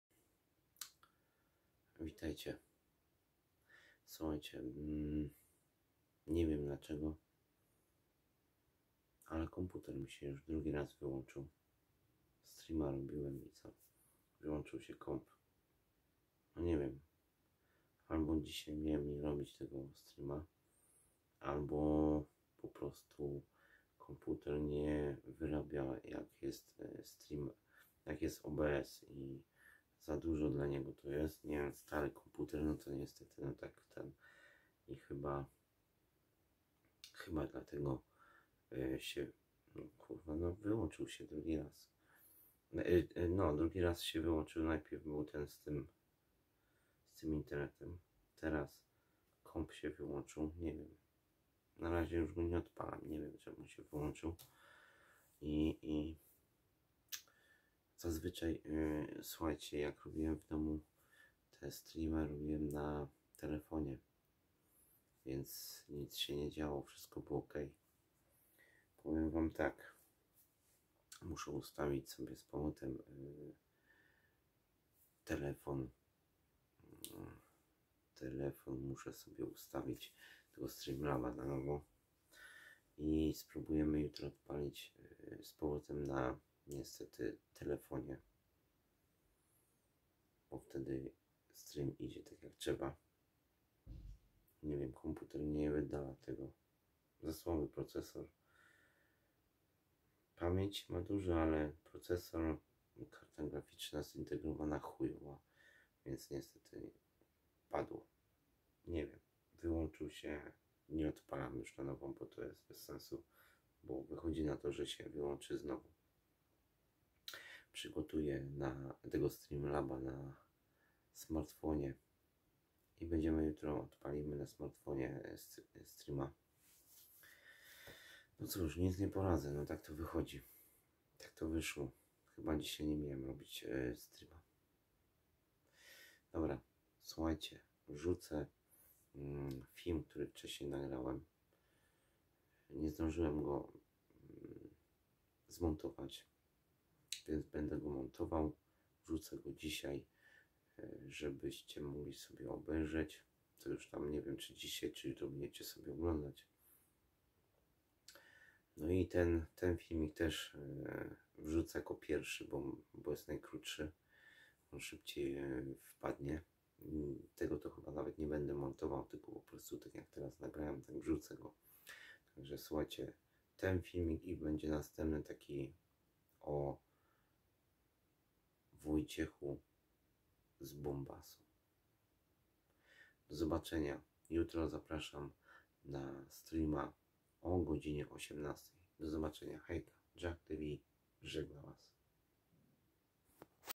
Przepraszam ale live nie wypalił bo coś z kompem się dzieje
Na rozwój kanału